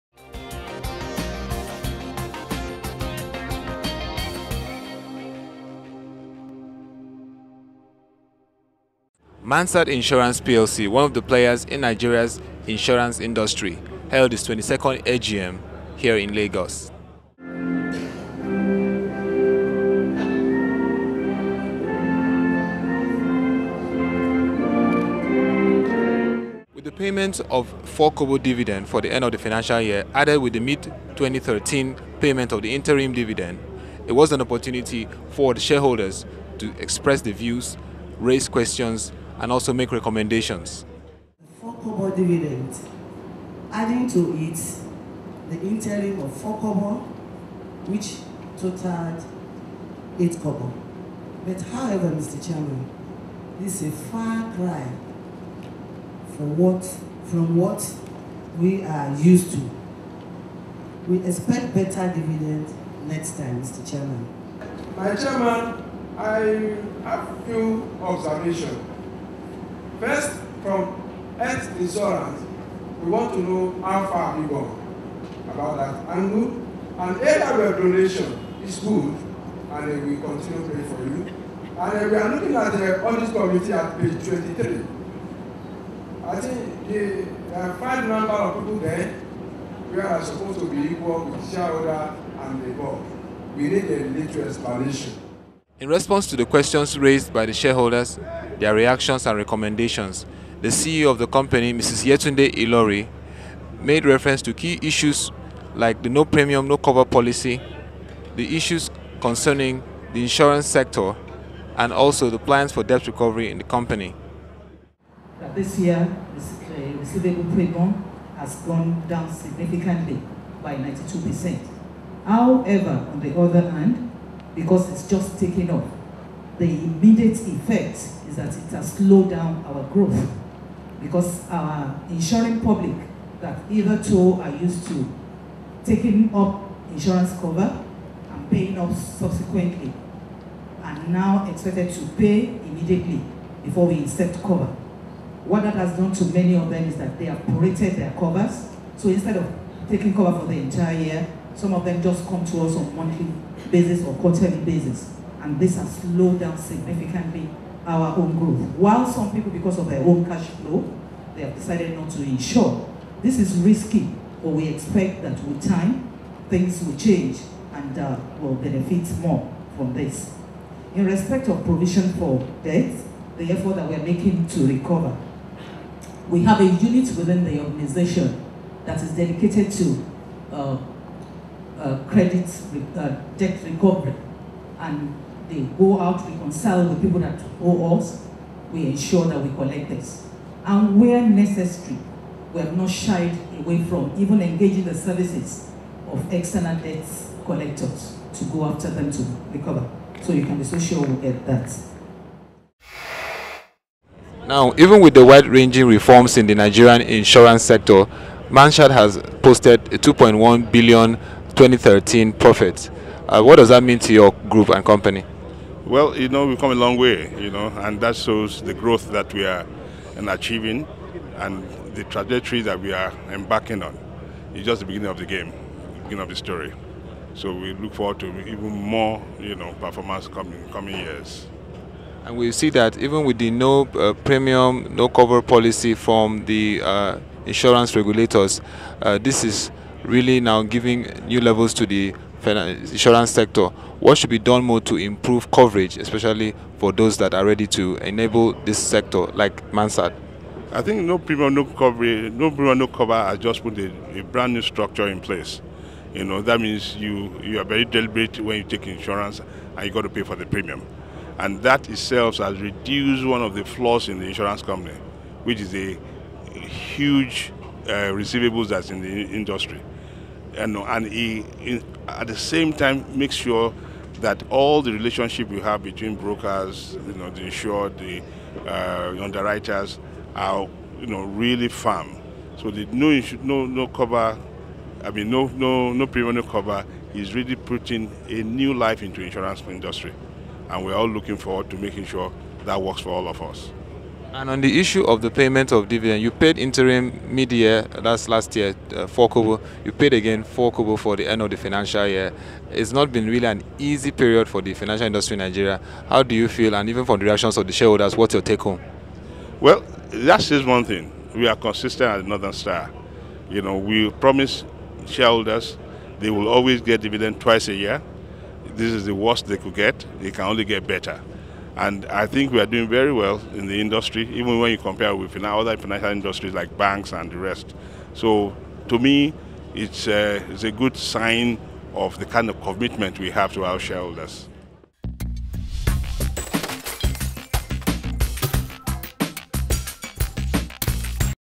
Mansard Insurance Plc 22nd AGM
Mansard Insurance Plc is one Insurance company that is proving, that the sector has a lot to contribute to the economy, if utilized effectively.

For its 2013 financial year, it was a sterling performance as it recorded a PAT of N2.1bl, thereby keeping its promise to shareholders and paying the dividend of 4 kobo after the initial interim payment of 4 kobo mid-2013, making it a total of 8 kobo.

Shareholders commended the efforts of the insurance company, but urged it to work assiduously to boost its Internally generated revenue, ensure prompt recovery of debt, reposition operations to adjust to the 'No Premium, No Cover' policy and explore the Health Insurance scheme.

In her response the CEO Mrs Yetunde Ilori assures the investment community, that  Mansard Insurance Plc is committed to upscaling its operations, and has explored the possibilities in the new insurance policy, established a special unit to tackle debt issues and restructured its subsidiaries to cater for the Health insurance scheme.